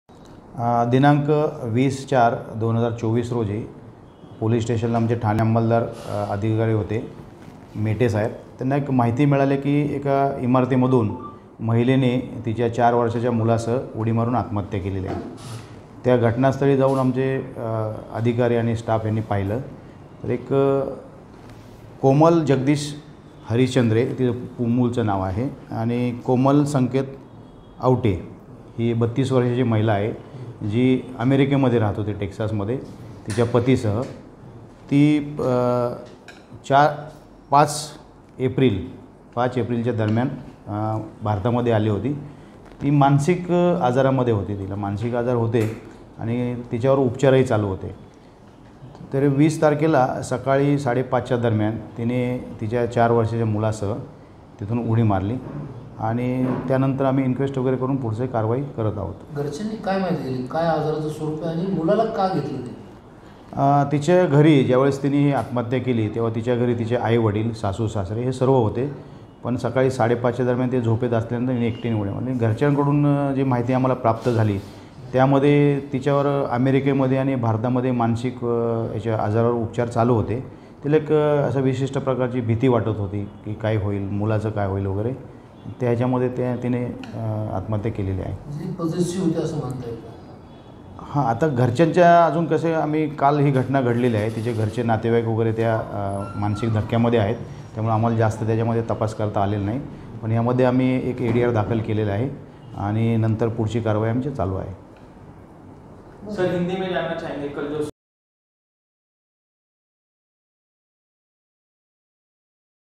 मेरिकेतून काही महिन्यांपूर्वी भारतात आलेल्या 32 वर्षीय महिलेने 4 वर्ष्याच्या मुलासह नवव्या...
Wakad Pune News | धक्कादायक ! अमेरिकेतून काही महिन्यांपूर्वी भारतात आलेल्या 32 वर्षीय महिलेने 4 वर्ष्याच्या मुलासह नवव्या मजल्यावरून उडी मारून केली आत्महत्या

कोमल जगदीश हरिश्चंद्र असे आत्महत्या केलेल्या महिलेचे नाव आहे. पिंपरी चिंचवडच्या वाकड मधील धक्कादायक घटना. सिजोफेनिया आजाराने ग्रस्त असलेल्या आईने आपल्या 4 वर्षांच्या मुलाला कडेवर घेऊन राहत्या इमारतीच्या नवव्या मजल्यावरून उडी मारून केली आत्महत्या. पिंपरी चिंचवड शहरातील वाकड भागात असलेल्या रीगालिया या उच्चभ्रू सोसायटी मधील घटना. घटनेत दोघांचा ही मृत्यू.